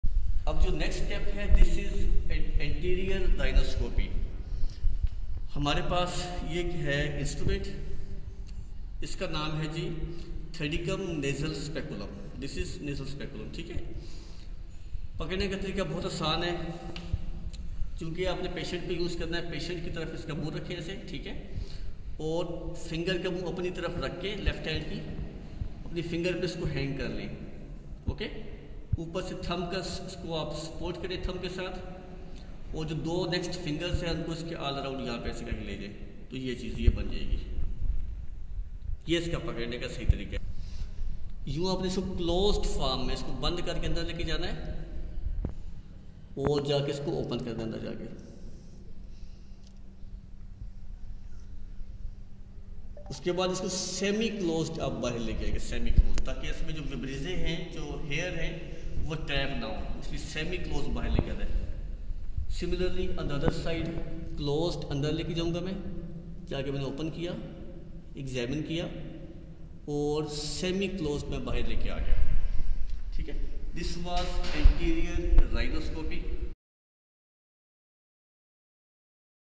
How to hold and use the Thudicum nasal Speculum.
A video for undergraduate Medical students of MBBS. This video demonstrates how to hold and use the Thudicum Nasal Speculum while performing anterior rhinoscopy.